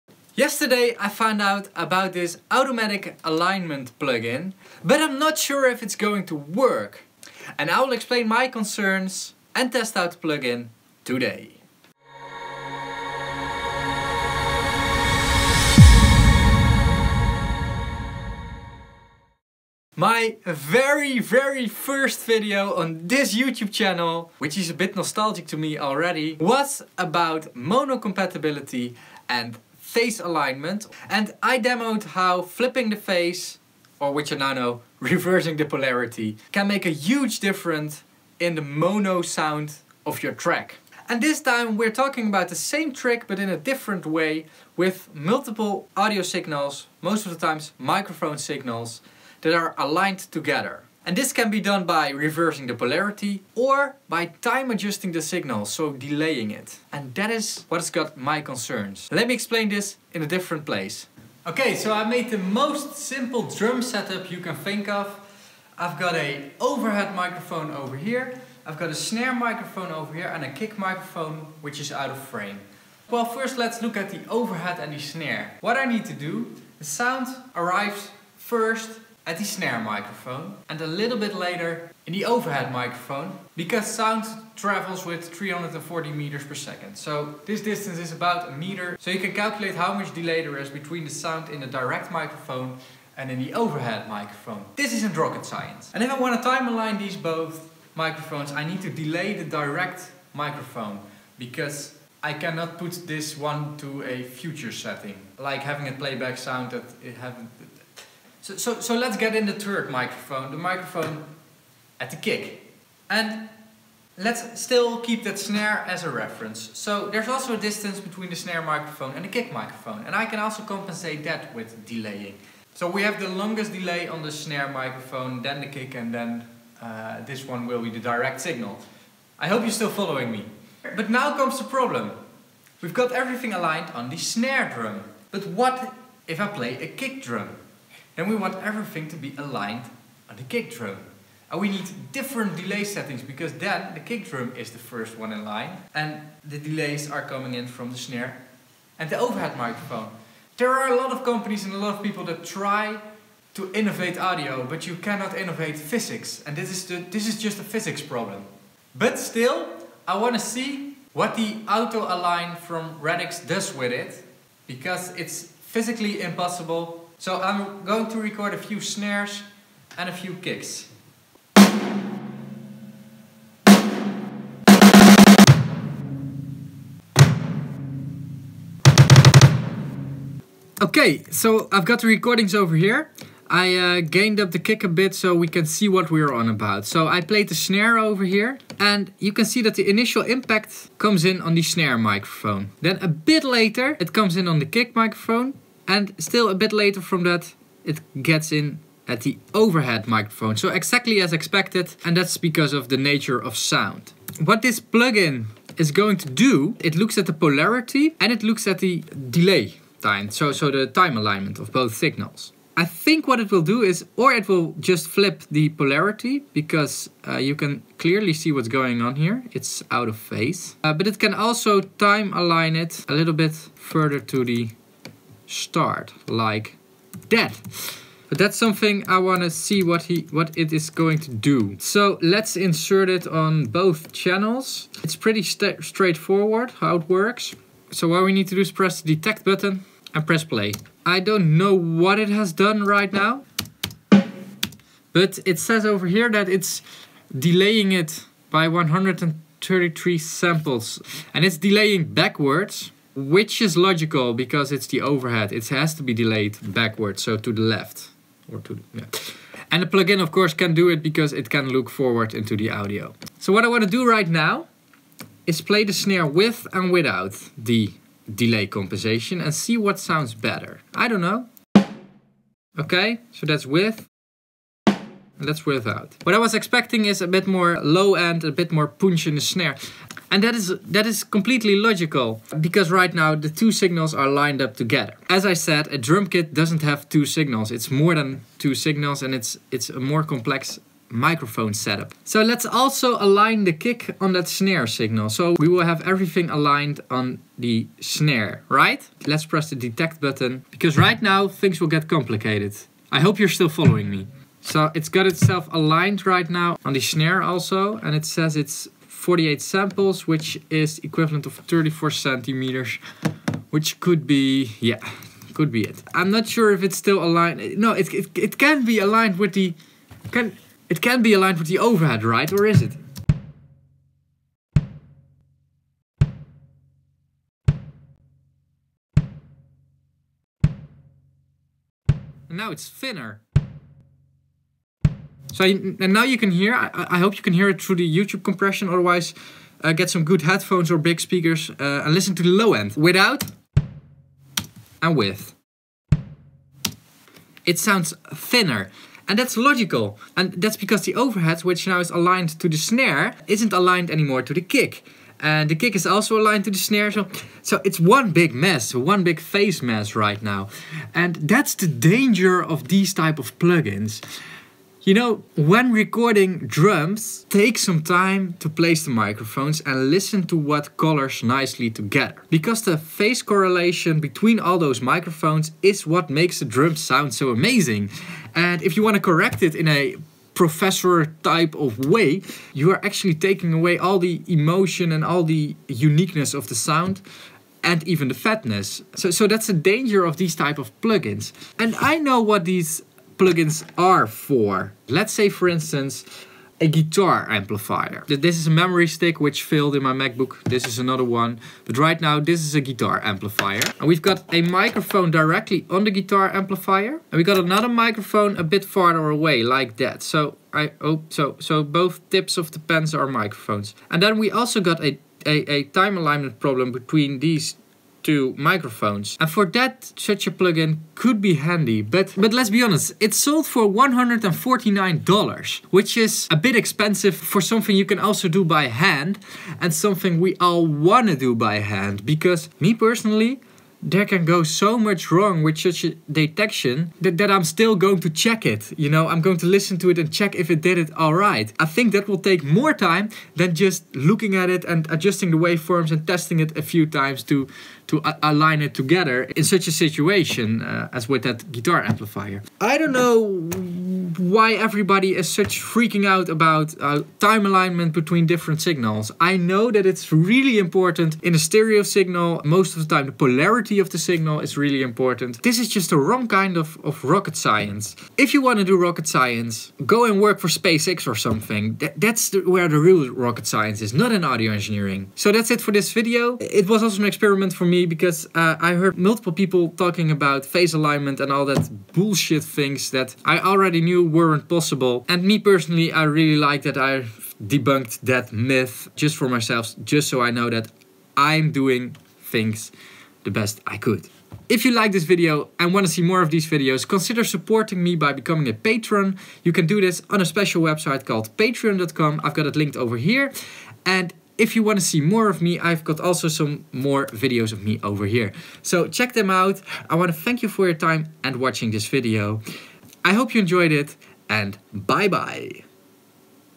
Should you PHASE ALIGN every channel?
Is it possible, or does it needs reinventing of physics?